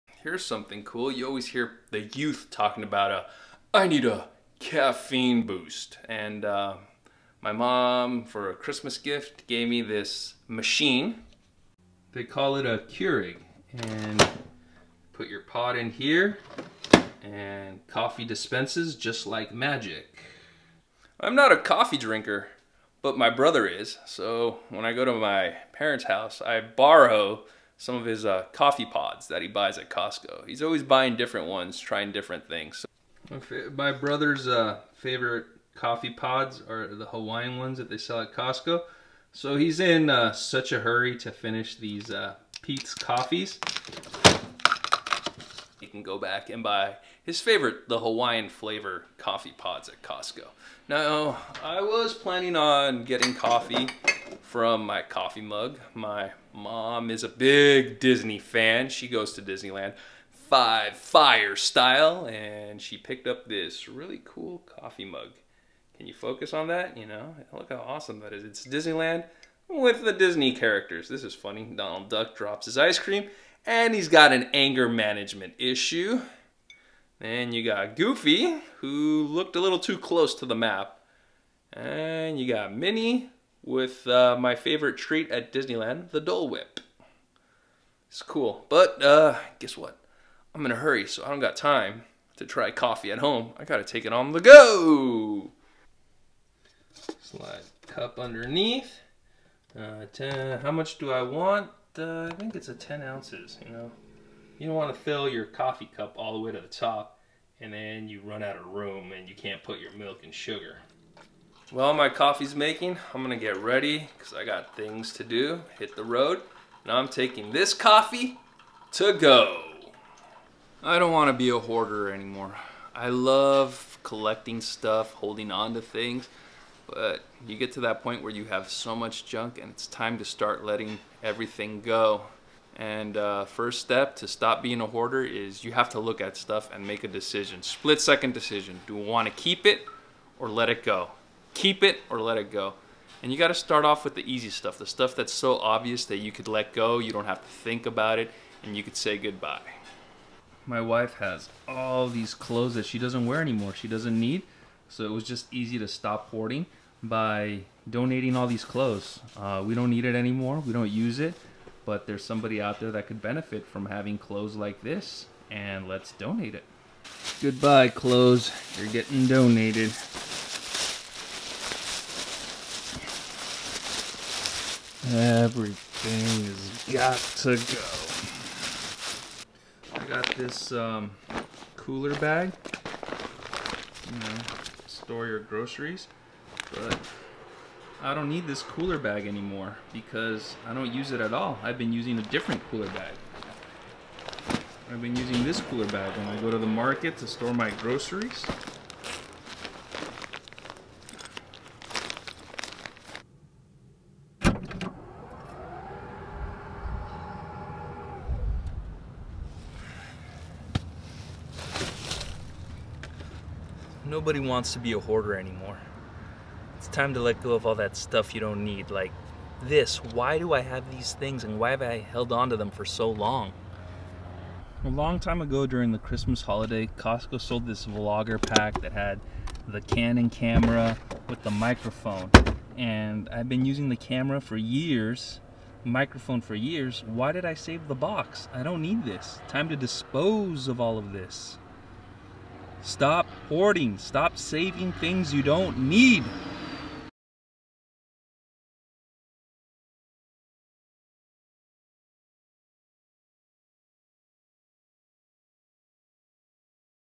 My Hoarding Journey part 3
Hello Everyone,

In this episode I take a coffee break before I continue on my Hoarding Journey.